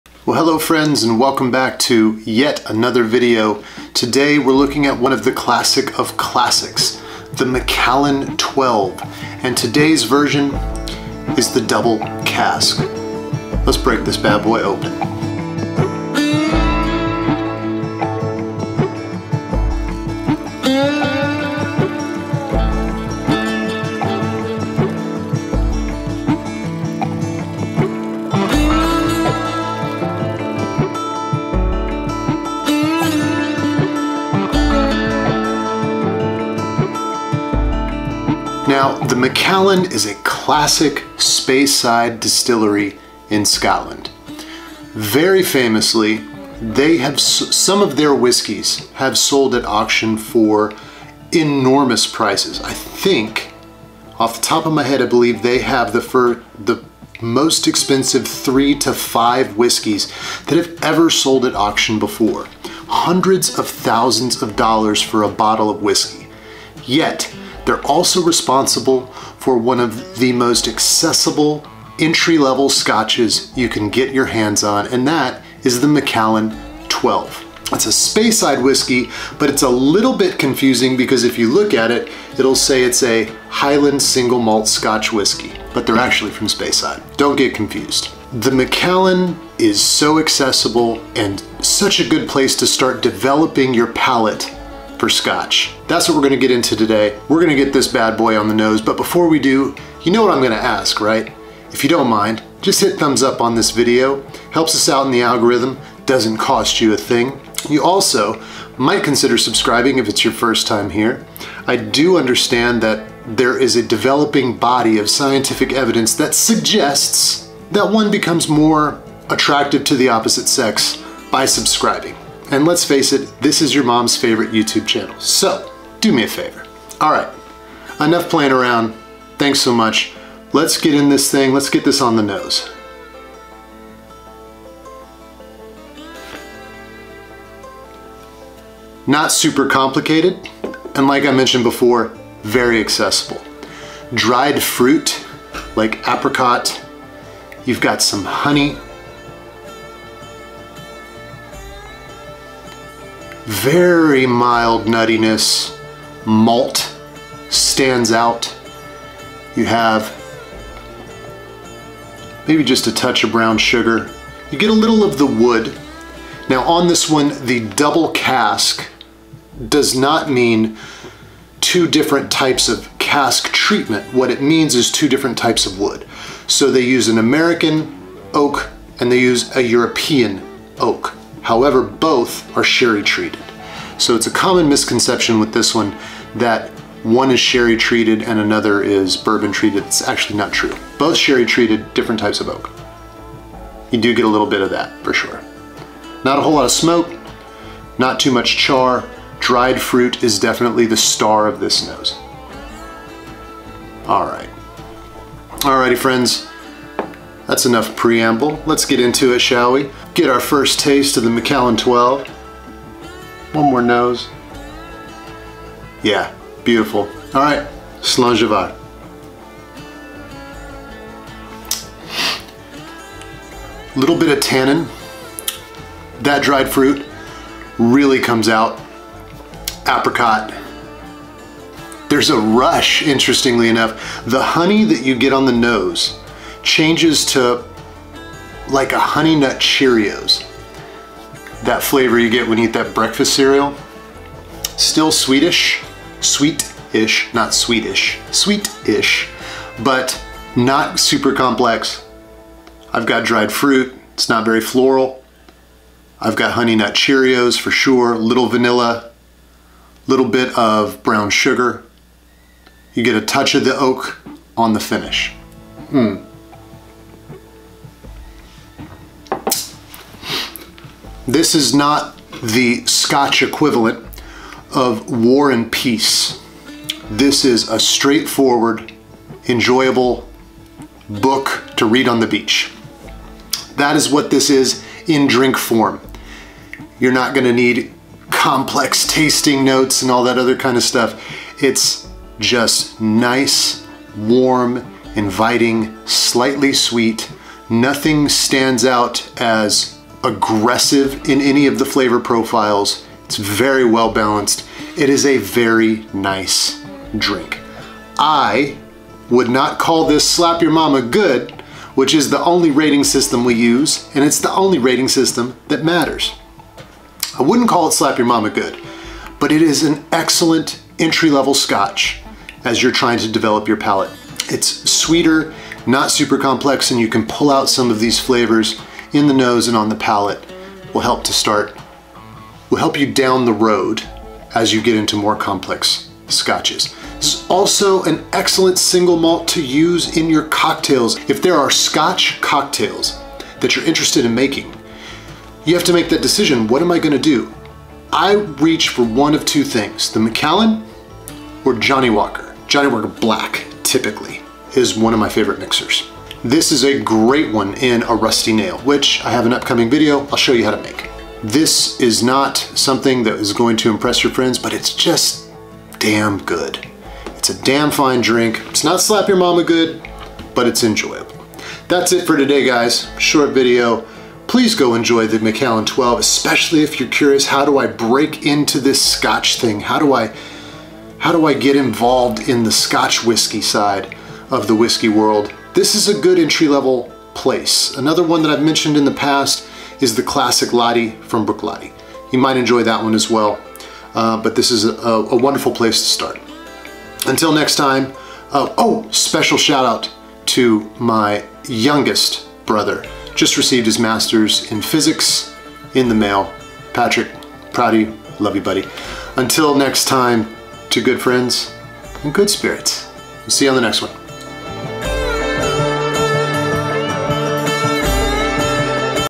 The MacAllan 12 Double Cask
scotch whisky The MacAllan 12 is one of those classic bottles of scotch. Inviting, simple, and yes...good. But is it slap your momma good?

**My Gear**

My main camera
My other camera
My drone